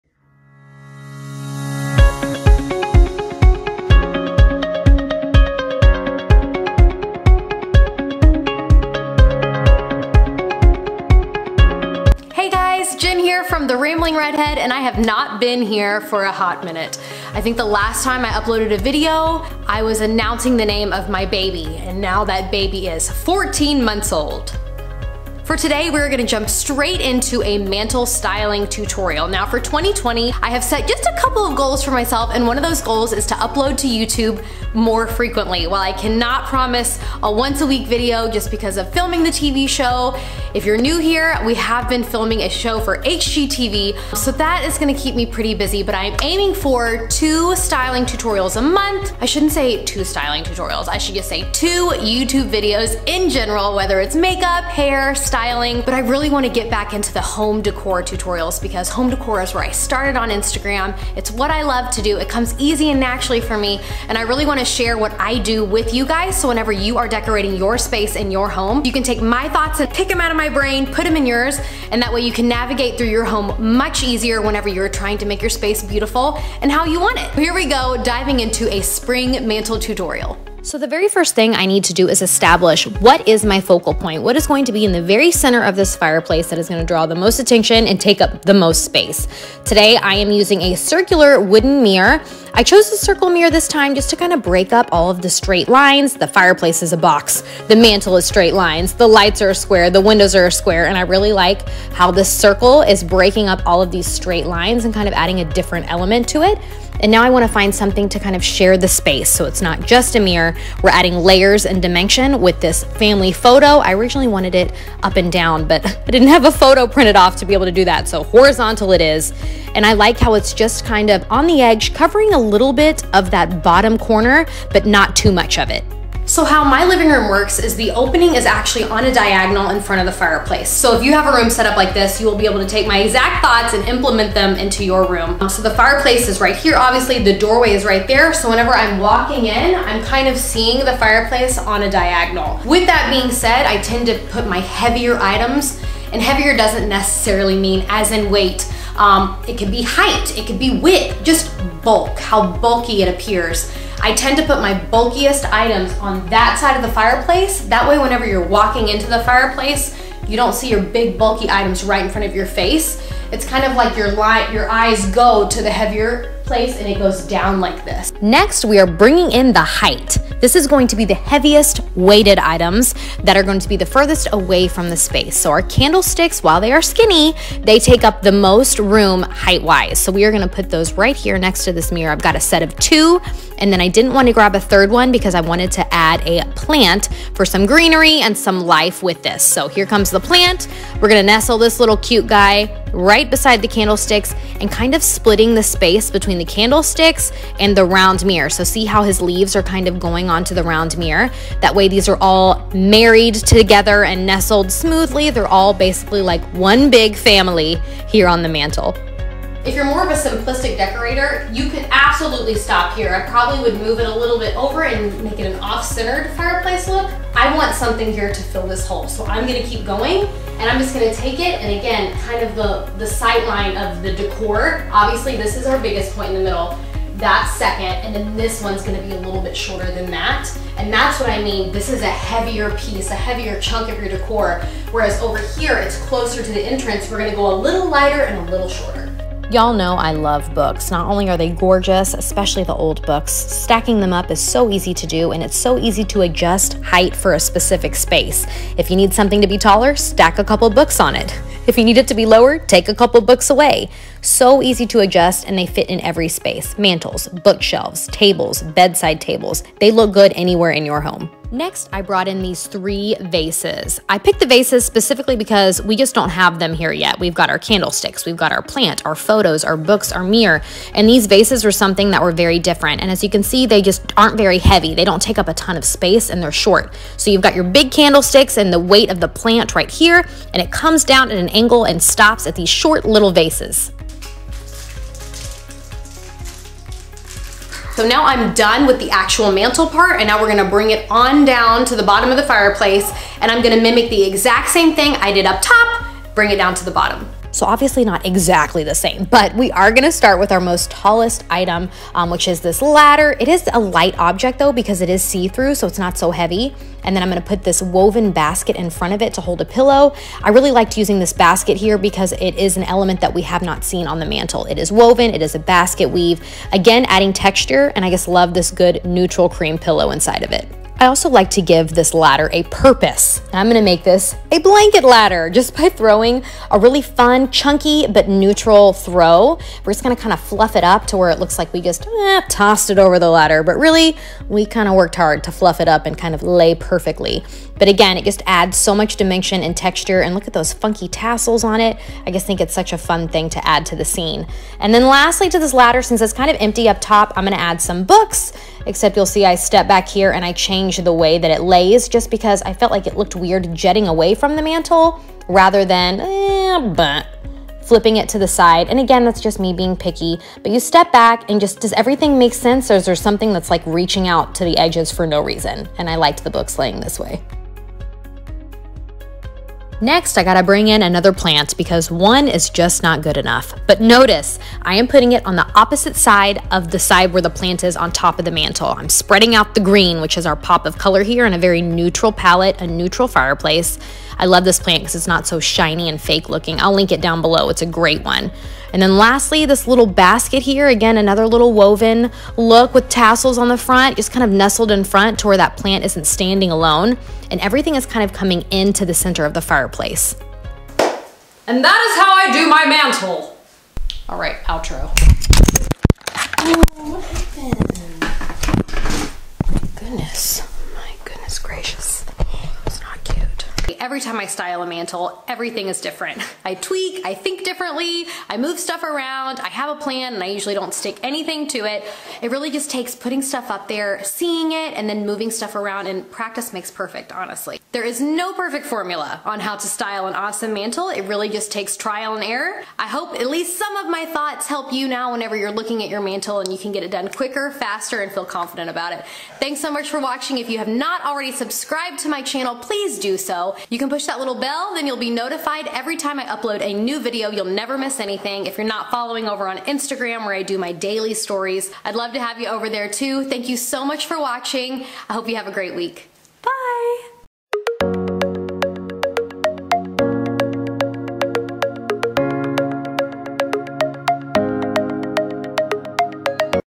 How To Style A Mantle! Spring Decorating Tutorial
How I style a mantle for any season!  Hopefully my thoughts and explanations in this video make decorating your own home a little bit easier! I am ALWAYS open to your ideas :)

For daily videos, tips and tricks follow on Instagram! @theramblingredhead

Items shown in this styling video:
Locust Wreath
Woven Vase
Glass Vase
7" Ceramic Vase
6" Ceramic Vase
5" Ceramic Vase
Gold Candle Sticks
Metal and Wire Basket
Black and White Picture Frame
Ceramic Jar Vase
Faux Plant in pot
White Ceramic Vase
Eucalyptus Wreath
Faux Fern Leaf
Glass Vase
Footed Gold Bowl
Wood Candle Sticks
Ceramic Vase with Tassel
Circle Mirror
Glass Tray